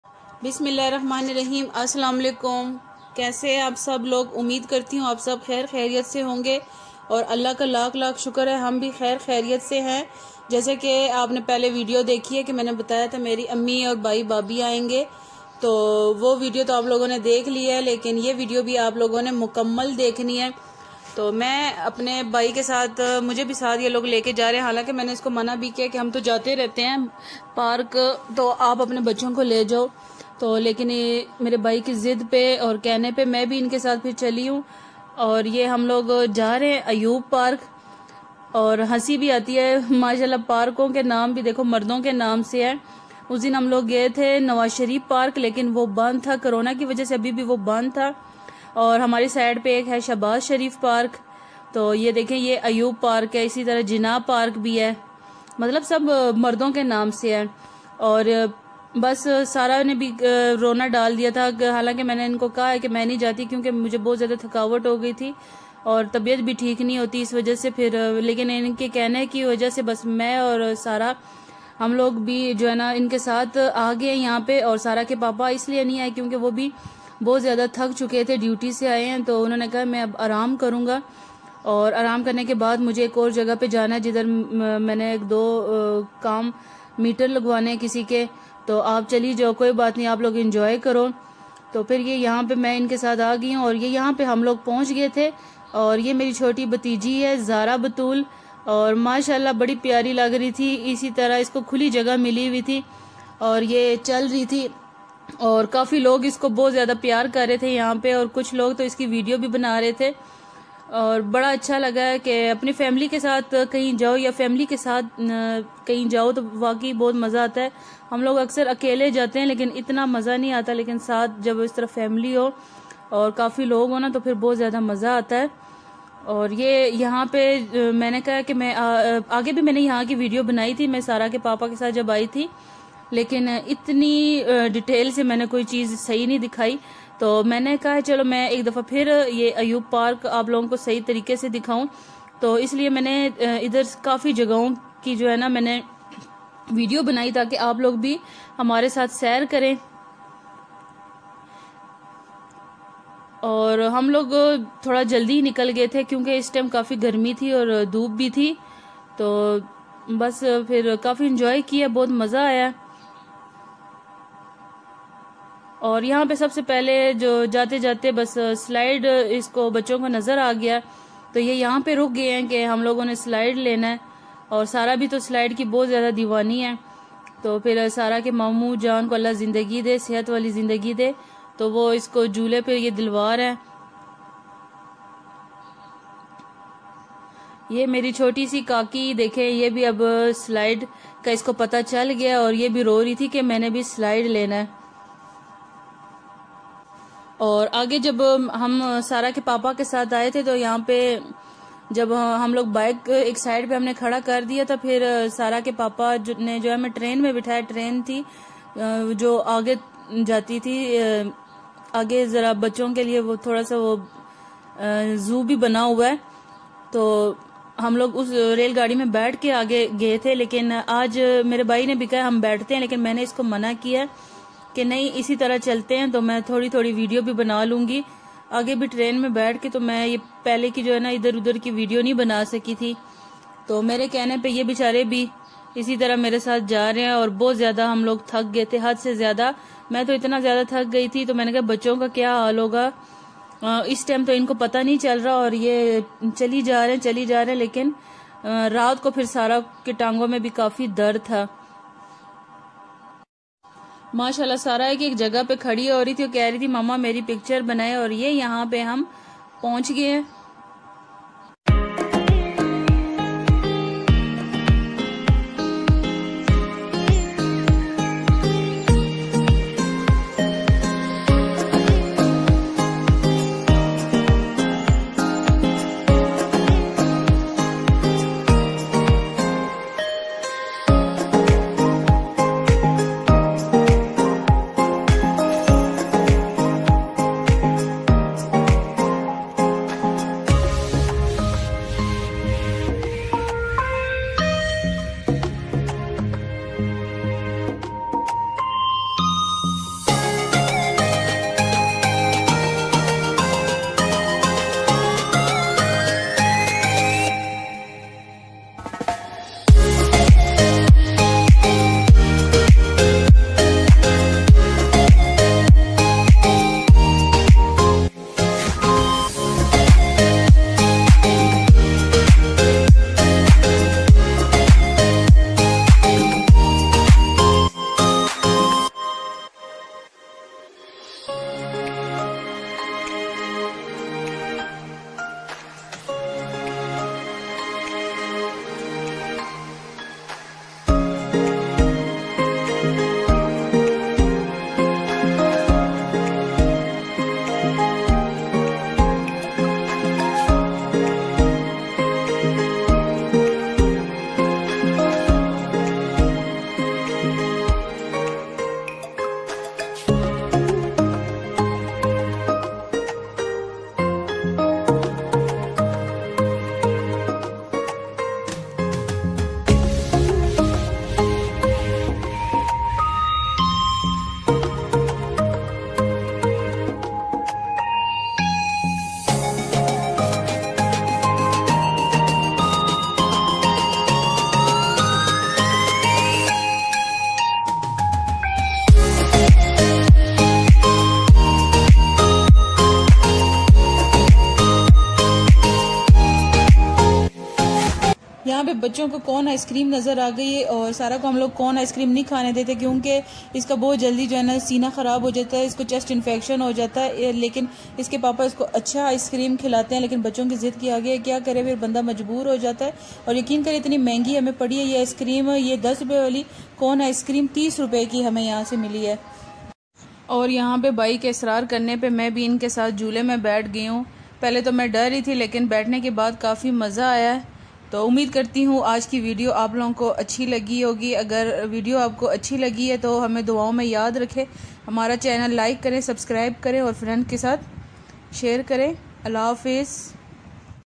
Family ky sath aik dafa phir Ayub national park rawalpindi aia⛲
Aslamualikum aumeed krti hun ap sb theek hongy aij ki vedio may hum aik dafa phir Ayub national park rawalpindi gy lakin iss dafa main Sara kay papa ky sath nhi gai main apni ami aur bhai ky sath gai. Kafi enjoy keya. Umeed krti hun aap sb ko vedio pasand ai ge duwa may yad rakain khuda hafez shukriya,

Ayub national park rawalpindi 1st vedio,

Meray ghr ayi ami jan. Kabli Palau video

Achay moasam main mood howa harab,